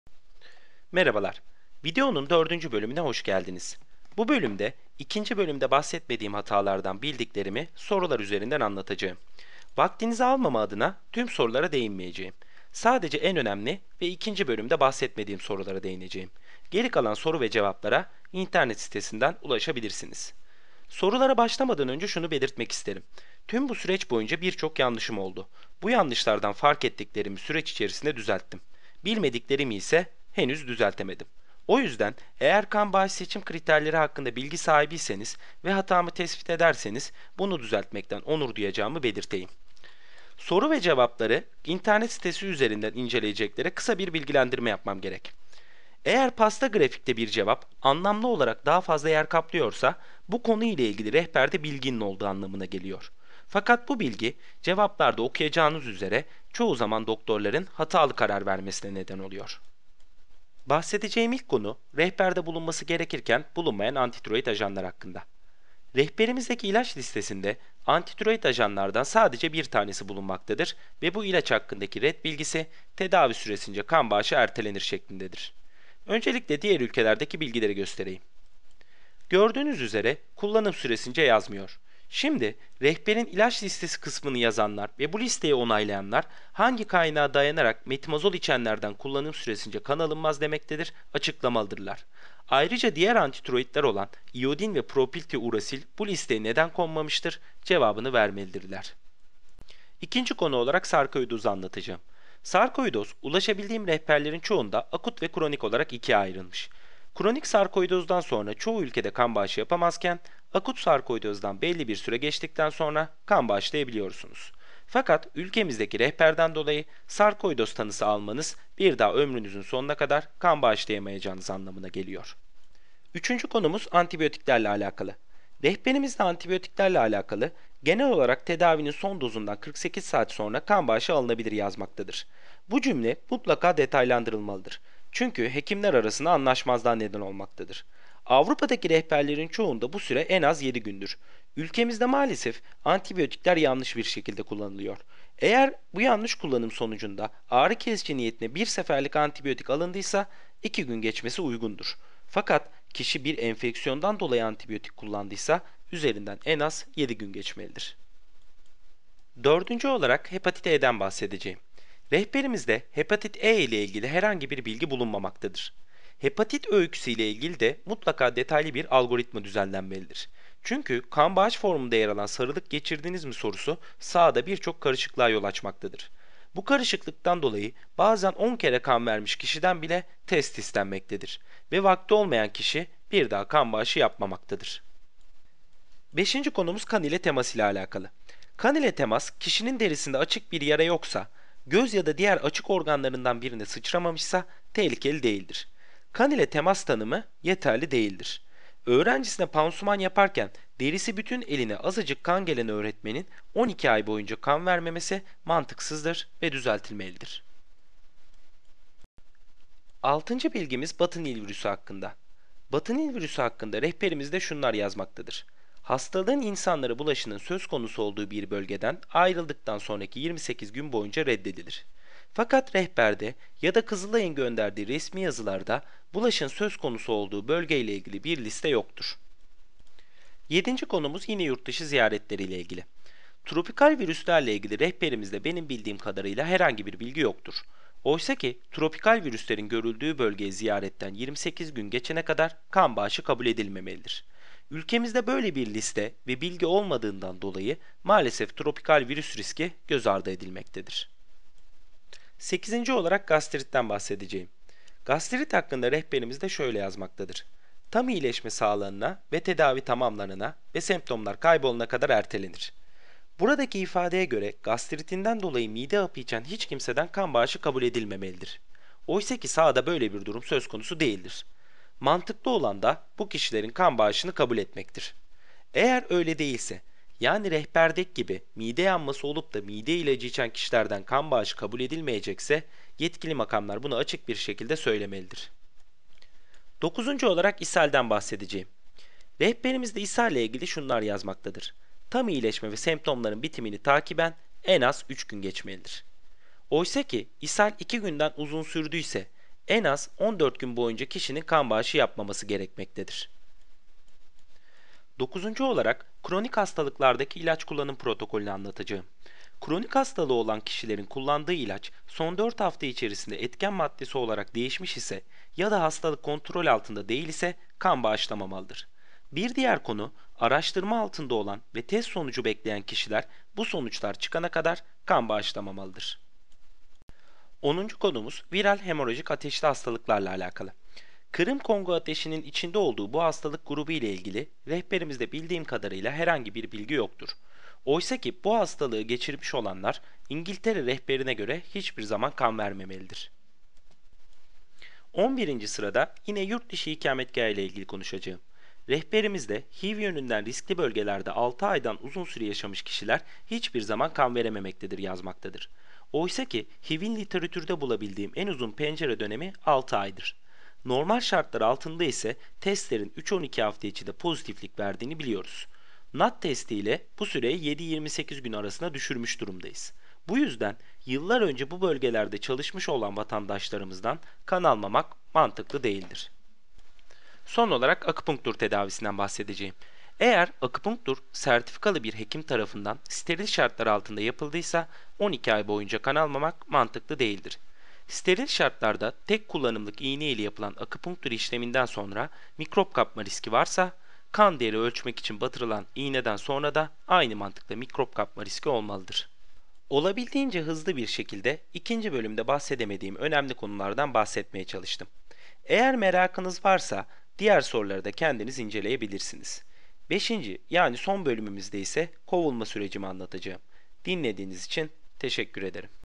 Bölüm 4 - Hatalı Kan Bağışcısı Seçimine Neden Olan Problemler (2) ve Vaka Soruları
Bu bölümde 2. bölümde üzerinde duramadığım ve bildiğim hataları anlatıyorum.